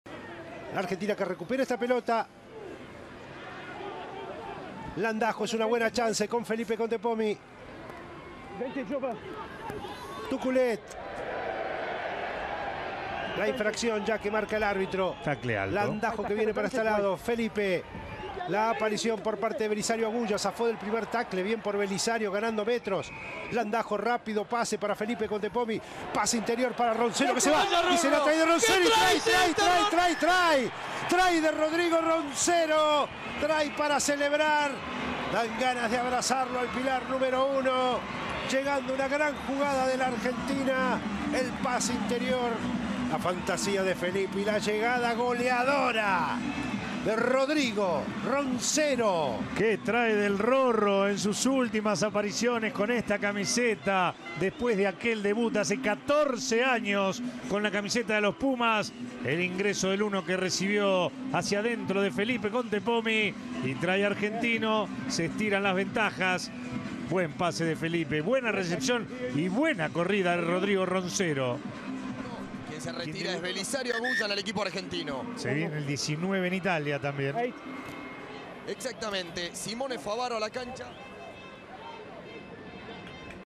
Try Rodrigo Roncero a Italia
Rodrigo Roncero se destacó en el triunfo de Los Pumas ante Italia.